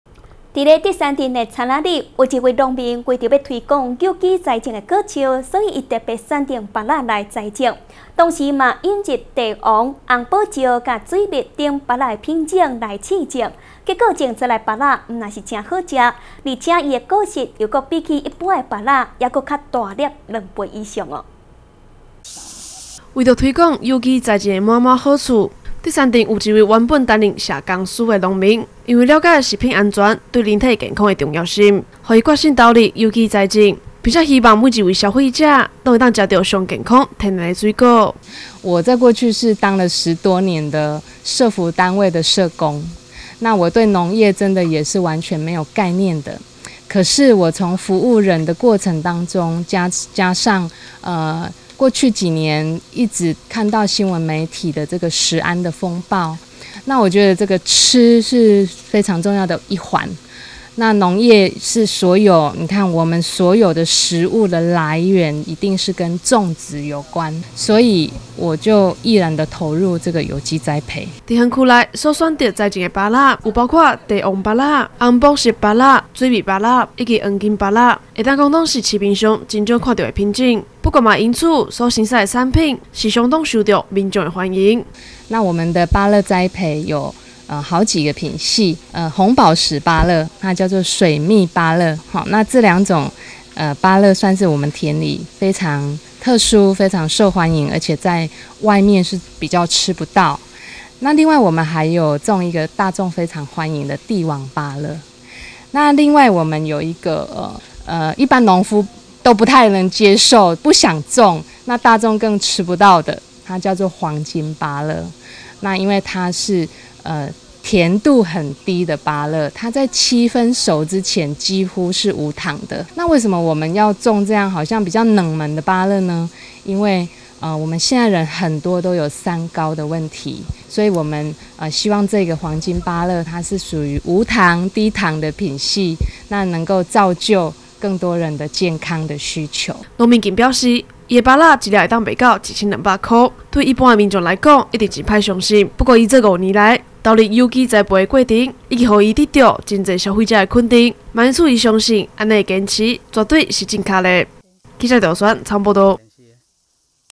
南投新聞 竹山有機芭樂
位在南投縣竹山鎮的有一位農民，為了要推廣有機栽種的果樹，特別選定以芭樂做為栽種目標，同時引入帝王芭樂、紅寶石巴樂及水蜜芭樂來試種，結果種出來的芭樂不但好吃，而且果實又比起一般的芭樂還要大了兩倍以上。